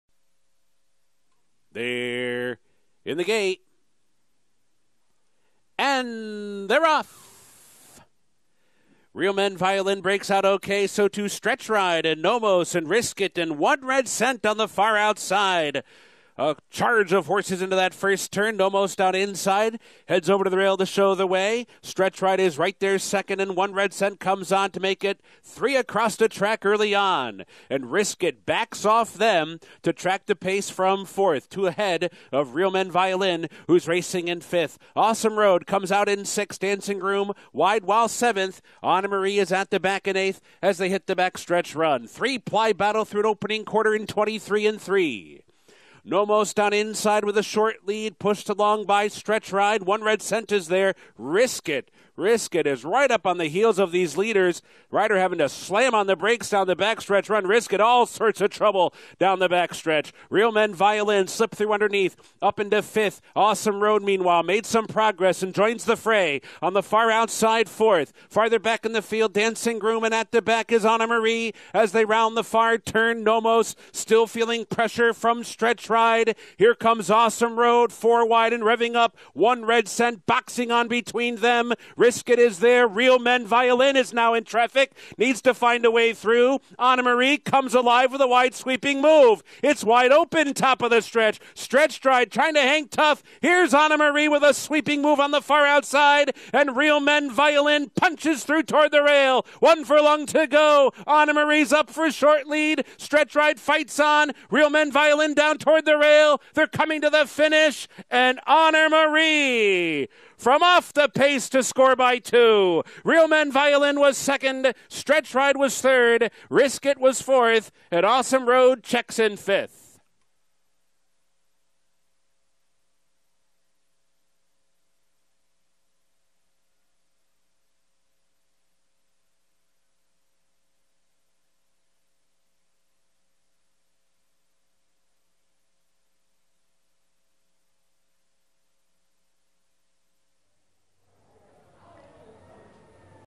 Honor Marie Wins The Kentucky Jockey Club Stakes - Derby Prep - Churchill Downs - 11/25/23
Honor Marie, with jockey Rafael Bejarano, wins the 2023 Kentucky Jockey Club Stakes at Churchill Downs Race Track. The mile and 1/16 race was won in 1:44.31. The Honor Code colt is trained by D Whitworth Beckman and earns 10 Kentucky Derby points for the win.  Runner up Real Men Violin and trainer Kenny McPeek picked up 5 points for the second place finish. Follow along all season as we keep an eye on all the 2024 Kentucky Derby contenders.